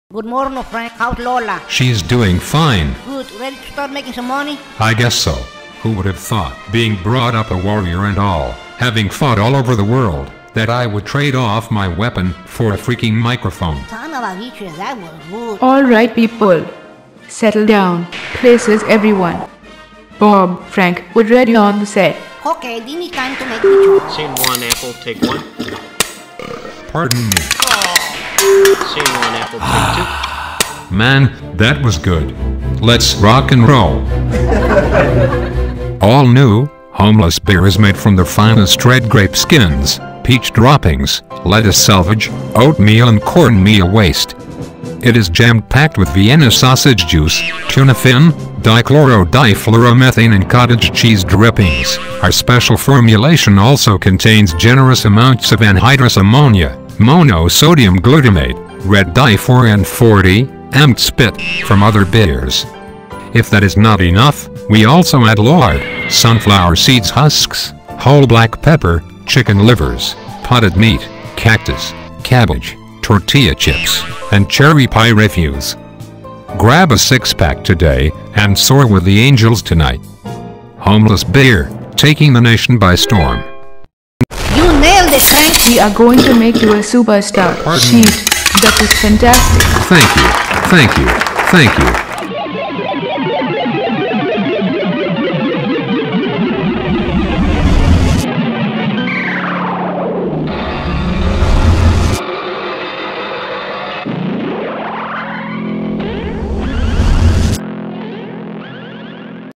Homeless Beer the Beginning
Frank got the job...he is the official spokesperson for Homeless Beer. He just finished his first video commercial for Lola's brothers advertising and video production company.
Lets see how long Frank can tolerate this job...he has had four different jobs since his return from "several wars" a couple of years ago - each one has lasted a couple of days.